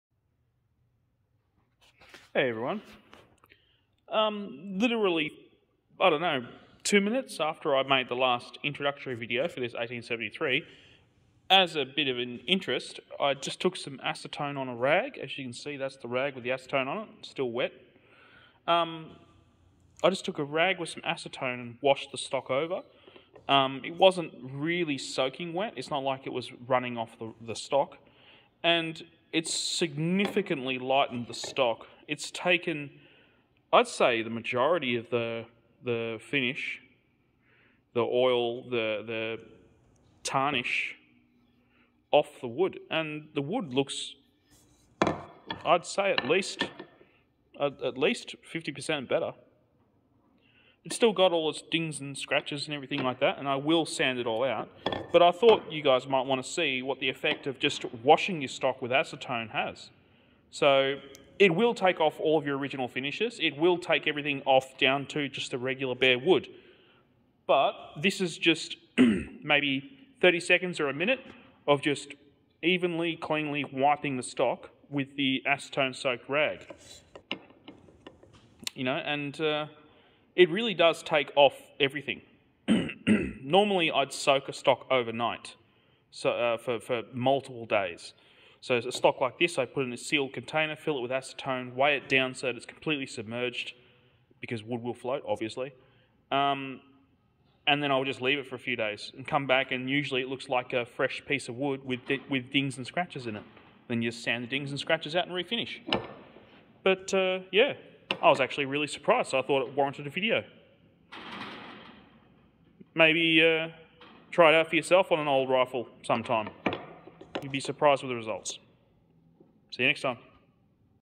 Winchester 1873 saddle ring carbine restoration part 2
Cleaning the stock.